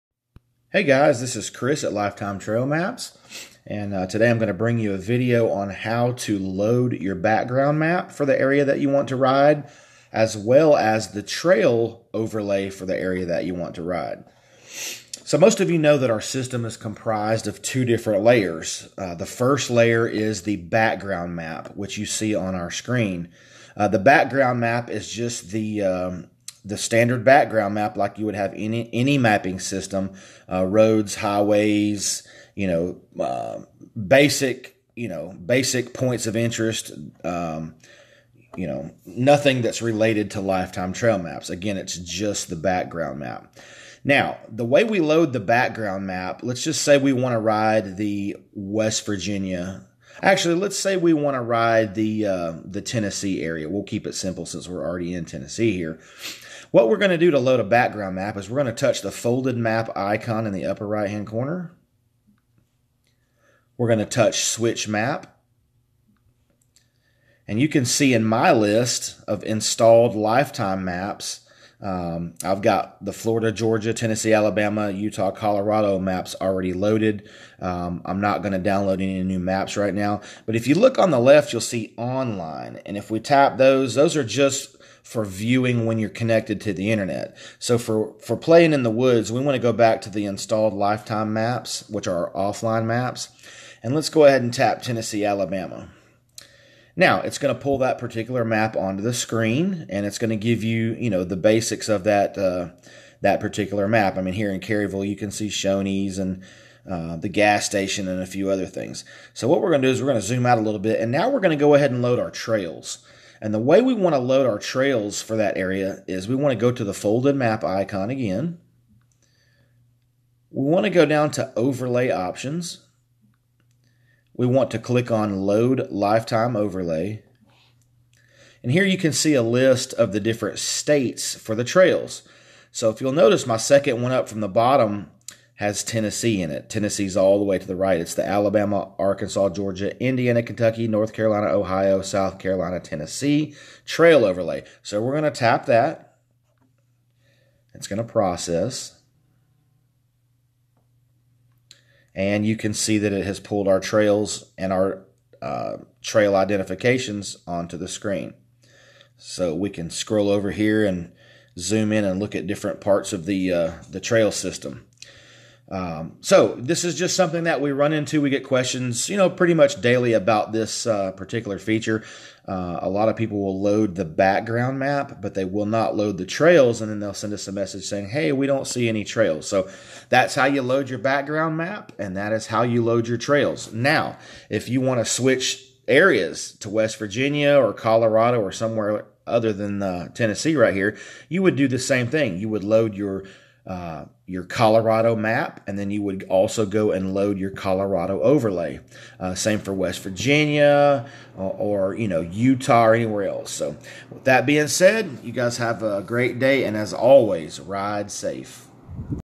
Loading Background Maps & Trail Overlays
This video describes how to load both layers of the Lifetime Trailmaps mapping system.

Layer 1 = Background Map
Layer 2 = Trail Overlay